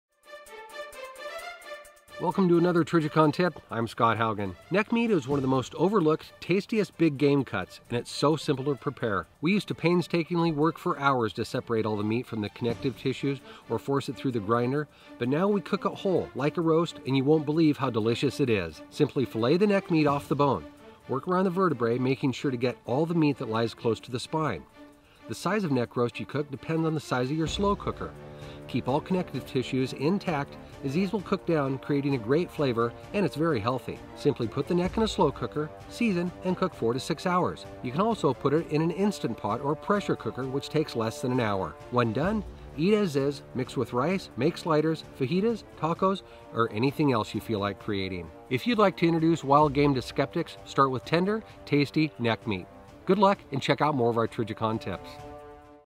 Harvesting Venison Neck Meat
Learn how to prepare one of the tastiest, but most overlooked, big game cuts. Scott Haugen explains how to harvest venison neck meat and gives examples of recipes that complement its flavor.